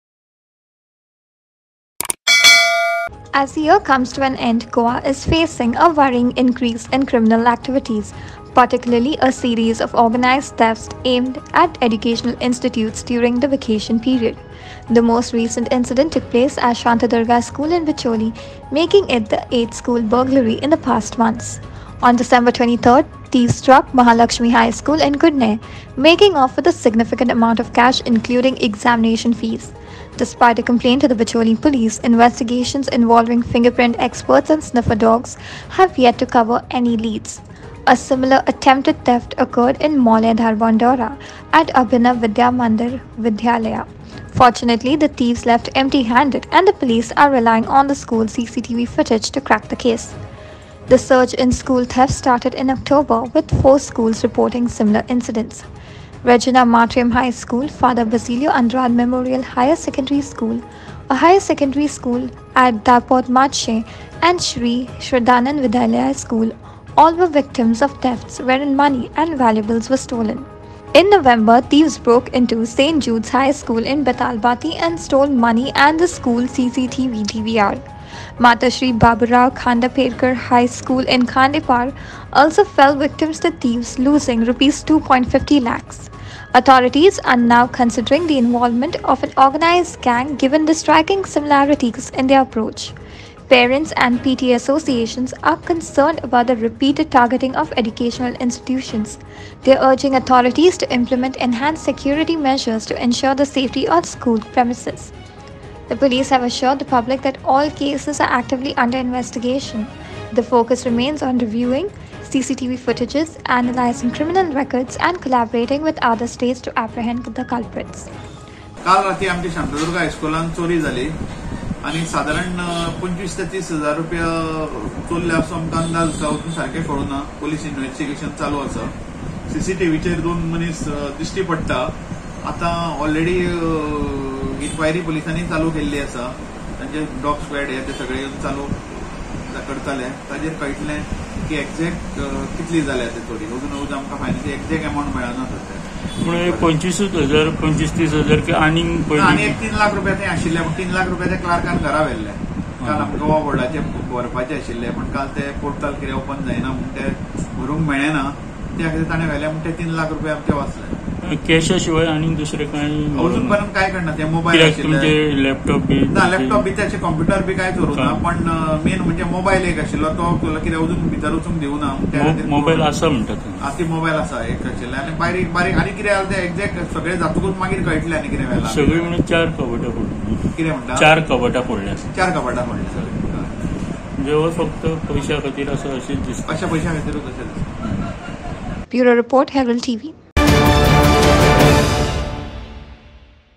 Shantadurga School in Bicholim Falls Victim to Burglary; Police Investigate Amidst String of School
Shantadurga School, Bicholim becomes the latest target as thieves strike again. This marks the eighth incident of school burglaries in the last two months. The Police are on site; further investigations are on.